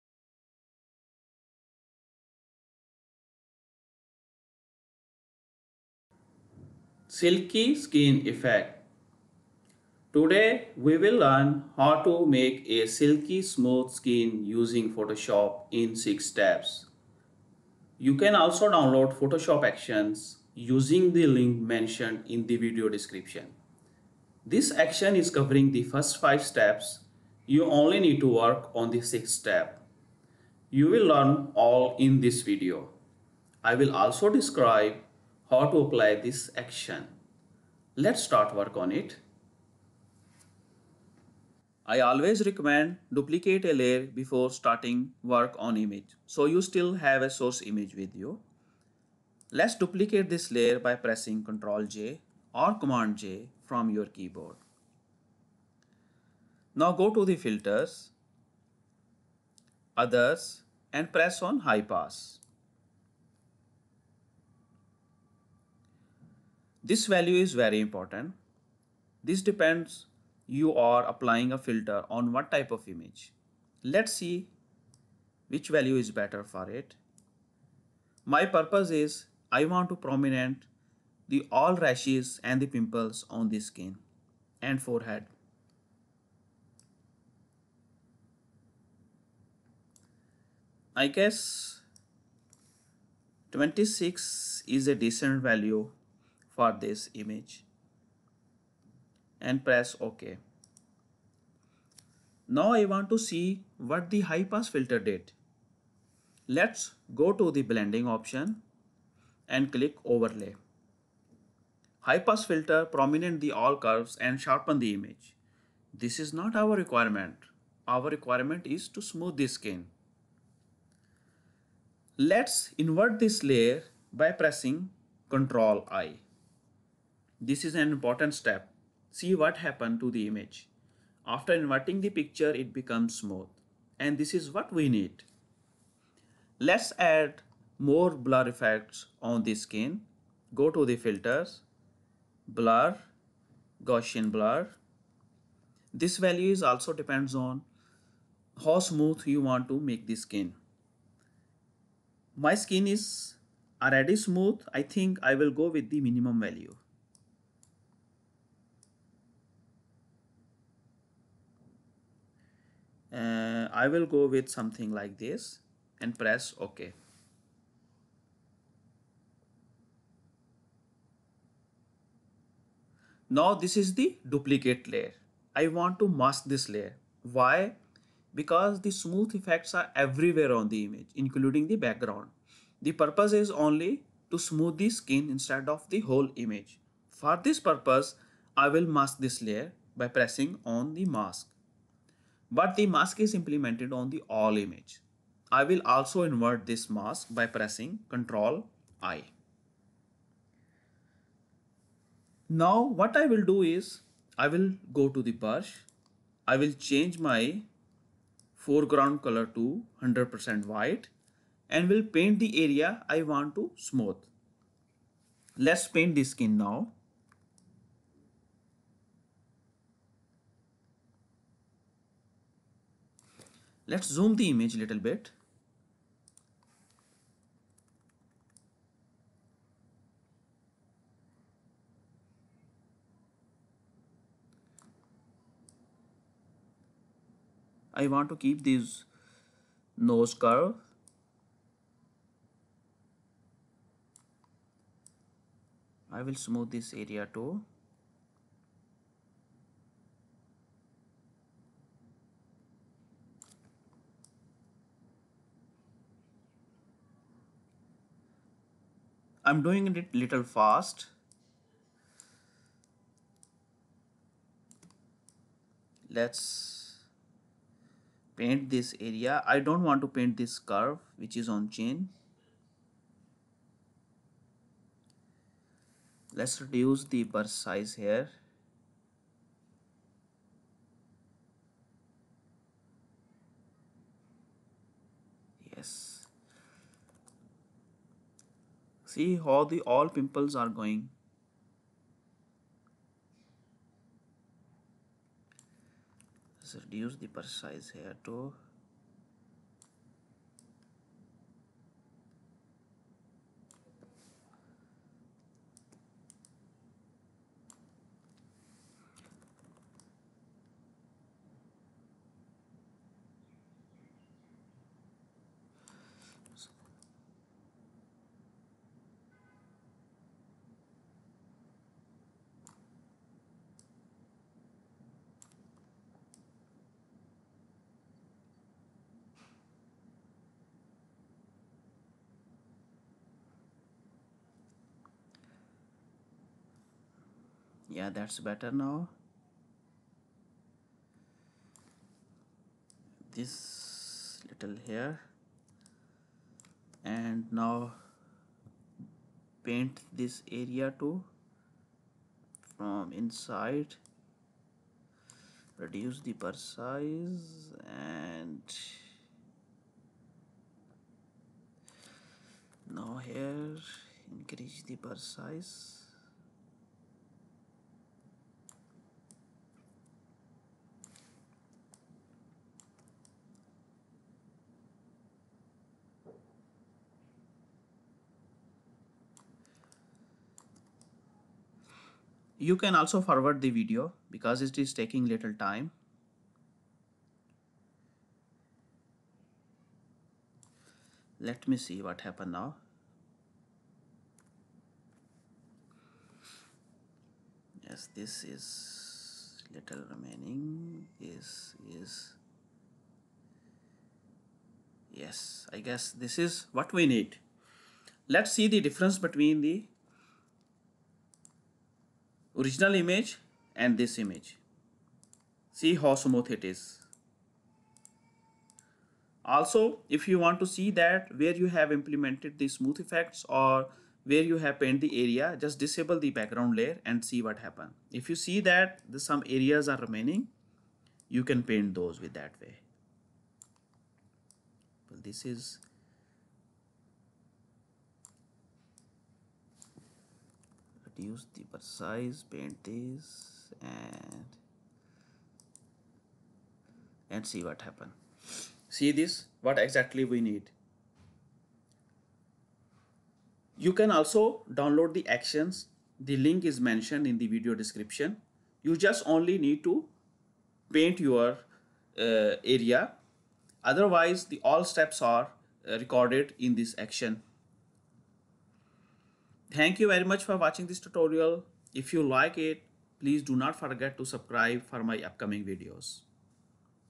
Make Soft Skin in Photoshop (Skin Retouching in Photoshop) in English
Professional Tutorial on how to make rough or wrinkled skin smooth and silky.
Today we will learn how to make a silky smooth skin in Photoshop in 6 steps.
You can also download Photoshop preset using the link mentioned in the video description.
This preset is covering the first five steps. You only need to work on the 6th step.
You will learn this all in this video.

Step 1: Apply high pass filter

Step 2: Apply blending mode

Step 3: Invert layer

Step 4: Add Blur

Step 5:  Add mask and invert

Step 6: Paint area you want to smooth